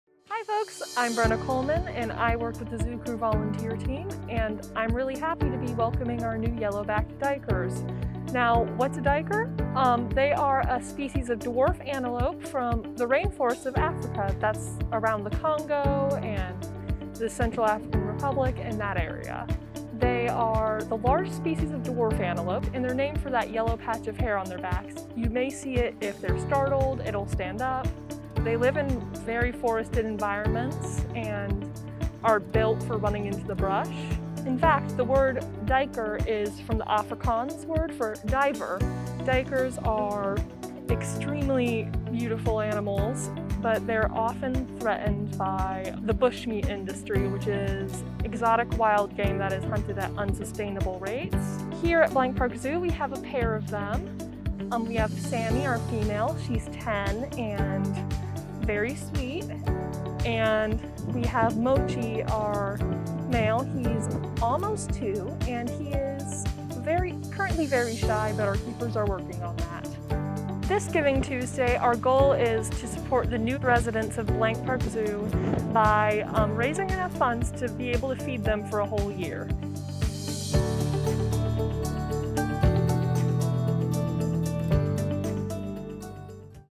Meet Blank Park Zoo's New Duikers
Meet Blank Park Zoo's newest species, the yellow-backed duikers, and learn how you can help them this Giving Tuesday.